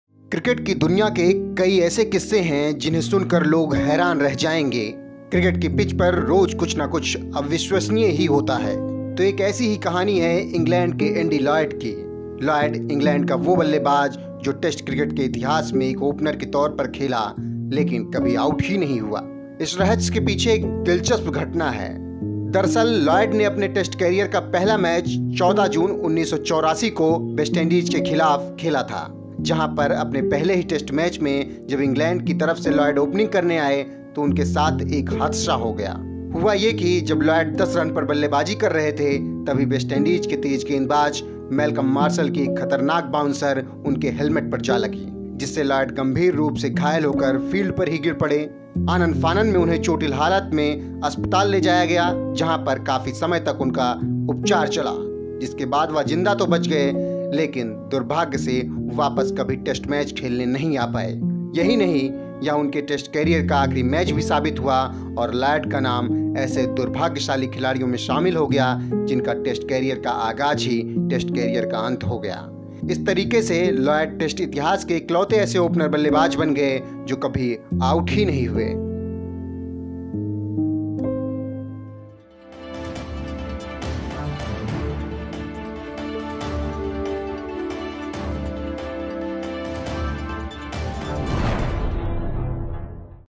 क्रिकेट इतिहास में कभी आउट नहीं हुआ ये बल्लेबाज
क्रिकेट के इतिहास में ऐसा पहले कभी नही हुआ कि कोई बल्लेबाज आउट ना हुआ हो, लेकिन इस बल्लेबाज ने ऐसा कर दिखाया है।